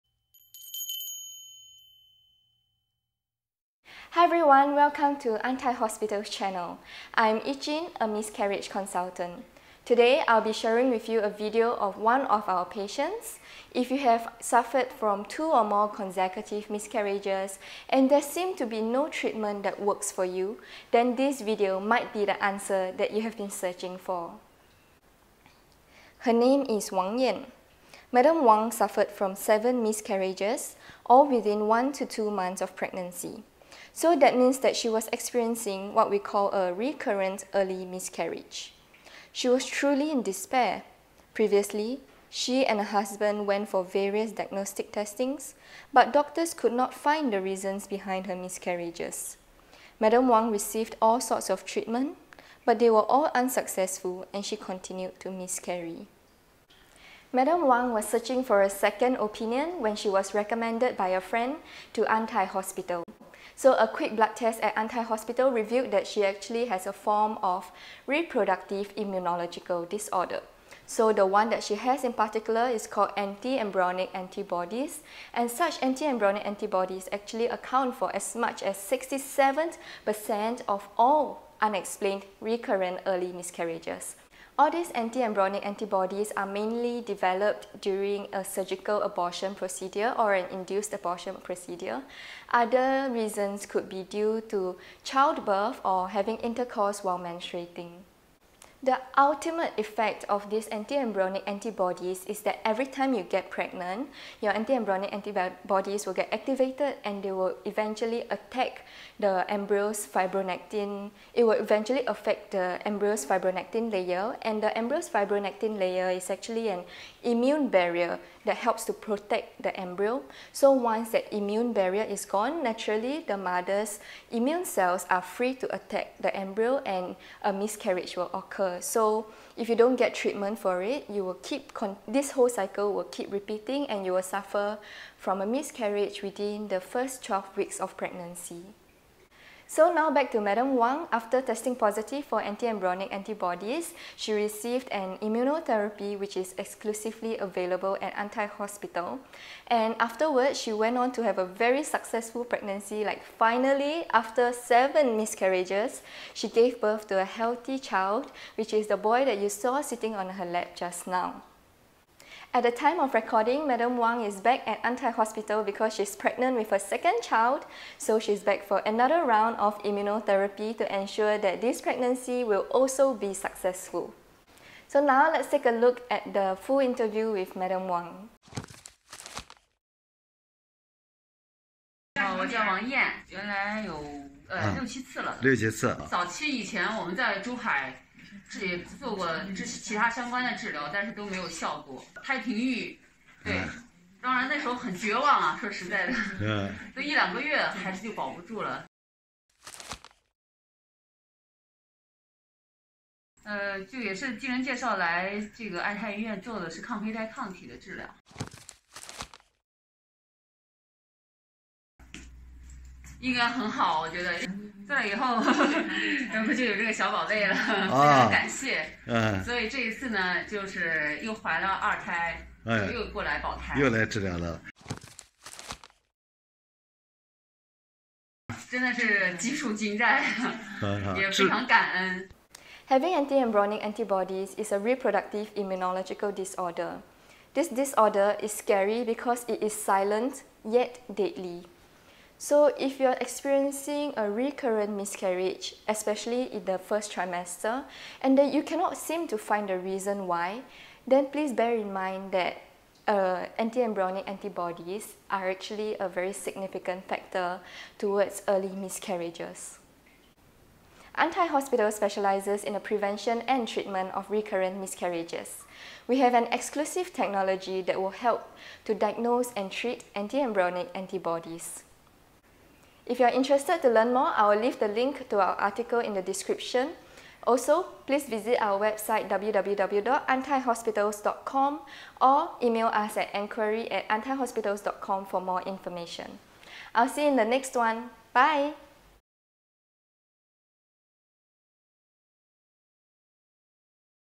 Unexplained Recurrent Early Miscarriage - Anti-Embryonic Antibodies w/ Testimonial | Antai Hospital
Accounting for 67% of unexplained recurrent early miscarriages, anti-embryonic antibodies are a significant contributor to miscarriages that is often overlooked.